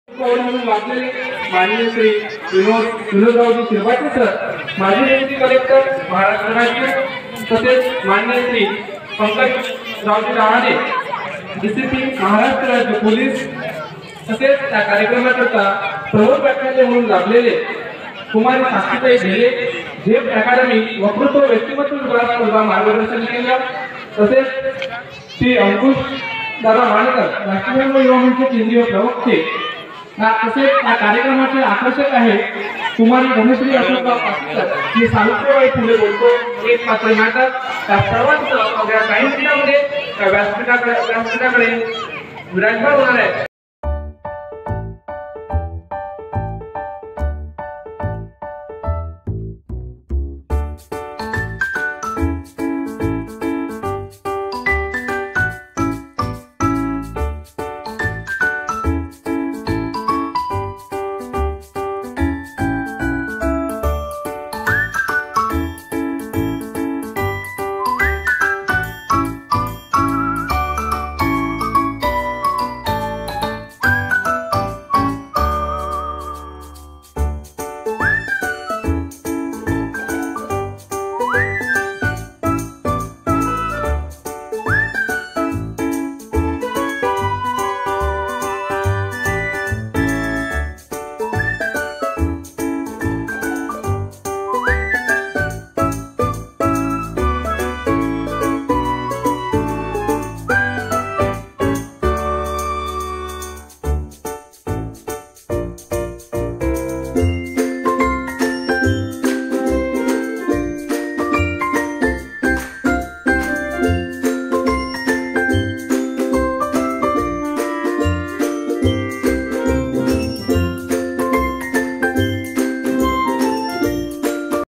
महात्मा ज्योतिबा फुले सावित्रीबाई फुले जयंती निमित्त अतिशय सुंदर 3D रांगोळी 🔥🔥...
सत्यशोधक अभ्यासिका व राष्ट्रधर्म युवामंच, अडगांव
वर्धापन दिनानिमित्त

मंगळवार दिनांक : ११ एप्रिल २०२३
वेळ : सायंकाळी ६.०० ते १०.०० स्थळ : बाजार चौक, अडगांव कार्यक्रमाला आपली उपस्थिती प्रार्थनिय आहे.

-: प्रमुख पाहुणे :--
मा. श्री. विनोद रा. शिरभाते (माजी डेप्युटी कलेक्टर महाराष्ट्र राज्य)
मा. श्री. पंकज डहाणे ( डी. सी. पी. महाराष्ट्र राज्य पोलीस)

--: कार्यक्रमाचे अध्यक्ष :--
मा. श्री. दिलीप अ. लोखंडे
अध्यक्ष (सावता शिक्षण संस्था) उपाध्यक्ष, महात्मा फुले को. ऑप. बँक, अमरावती

--: प्रमुख व्याख्याते :--
कु. साक्षीताई च. भेले (झेप अकॅडमी वक्तृत्व व्यक्तीमत्व विकास स्पर्धा मार्गदर्शन केंद्र)
चि. अंकुश र मानकर (राष्ट्रधर्म युवामंच केंद्रिय प्रवक्ते)

--: कार्यक्रमाचे आकर्षण :--
कु. धनश्री अशोक काशीकर
( मी सावित्रीबाई फुले बोलते ) - एकपात्री नाटक

--: इतर उपस्थिती :--
सौ. मंगला वि. लोखंडे ( सरपंच ग्रामपंचायत, अडगाव )
श्री. किशोरराव म. आमले ( उपसरपंच ग्रामपंचायत, अडगाव )
साबळे गुरुजी ( सेवानिवृत्त शिक्षक )
विशाल भा. तडस पोलीस पाटील (अडगाव / कसमपूर)

* विनीत *
सत्यशोधक अभ्यासीका समिती,अडगांव
राष्ट्रधर्म युवामंच, अडगांव
समस्त युवा गावकरी, अडगांव